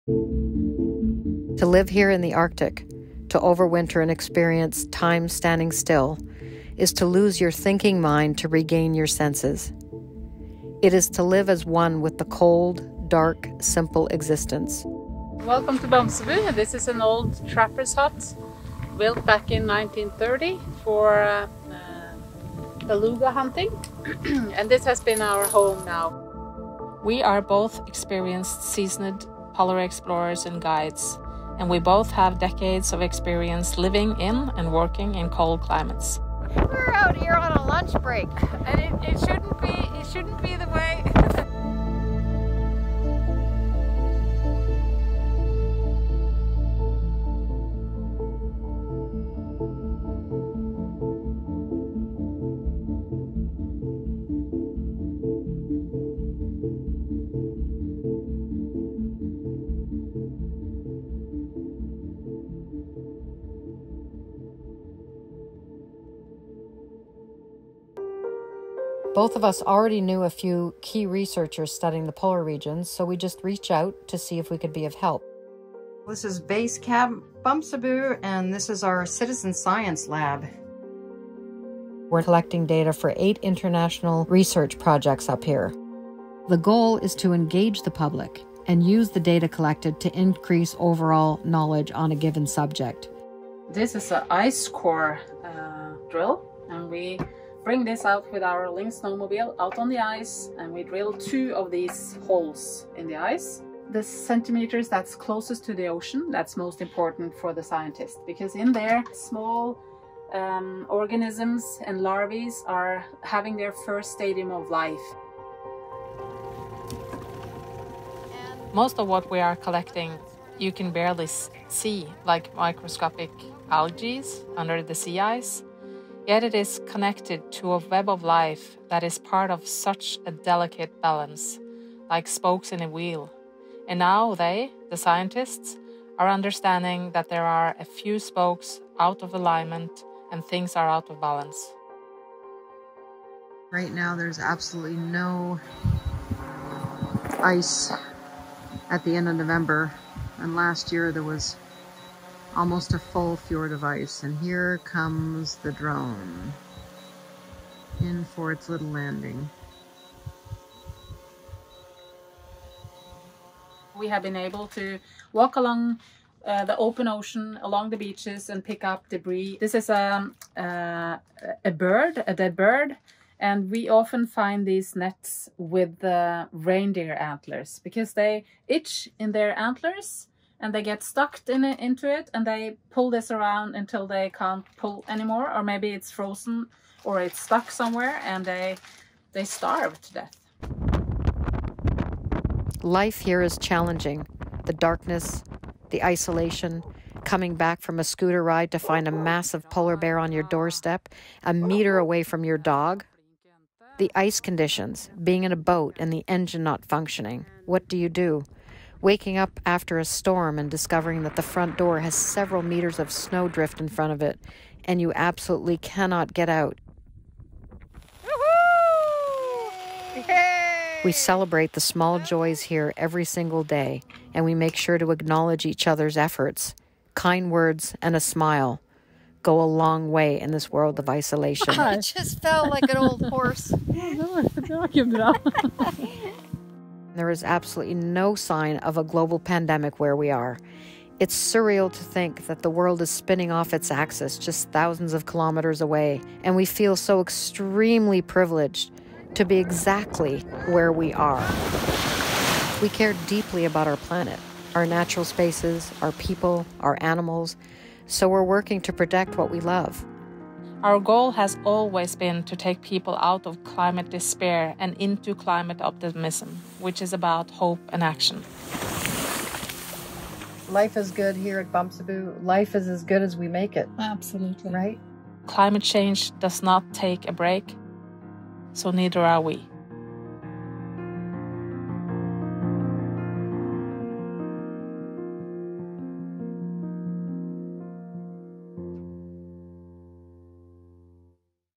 Meet Two Arctic Explorers Turned Citizen Climate Scientists | TIME
In late 2019, expeditioners and guides Hilde Falun and Sunniva Sorby went to Norway’s remote Svalbard archipelago to complete a long-term goal of being the first female team to over winter in the Arctic. But the pair’s planned return home to mainland Norway coincided with the start of the COVID-19 pandemic and pretty quickly they found themselves stranded. There had been plans for a ship carrying friends and family to come and collect them as the ice began to melt in March, but travel restrictions got in the way, and they couldn’t come home until September.

So instead, they spent the winter and much of spring up until May in an isolated, tiny wooden hut high up in the Arctic circle, surrounded by winter darkness. There was a definite upside, though, at least for the global scientific community: just as the two were stuck in Svalbard, fieldwork by climate scientists and researchers came to a standstill, as those who would normally travel to the Arctic to monitor the levels of melting ice as the region transitions from winter to summer were stuck at home. Faced with prolonged Arctic isolation as the rest of the world dealt with a growing pandemic, Falun and Sorby started collaborating with the scientists unable to travel, collecting data and samples to aid their research. Their work proved so useful that, after they finally did get to go home, they came back to their little hut for a second winter at the end of 2020.